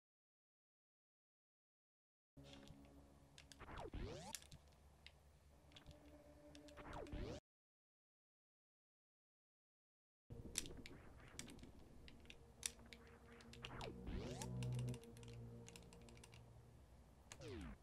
Maybe the fastest way to do moonfall parlor?
Sometimes the fastest strat was right infront of us the whole time.

Found this like a month ago, but didn't connect to emu to test speeds until today. I'm not sure if this has already been known about, but if you spin jump and do some really precise right to left stuff, you can actually make is between these platforms and save like 5-7 frames I think? You can also aim down and graze the silver beam and get a corner boost if you're good enough.

This isn't super difficult, but it seems really inconsistent. Still interesting, though.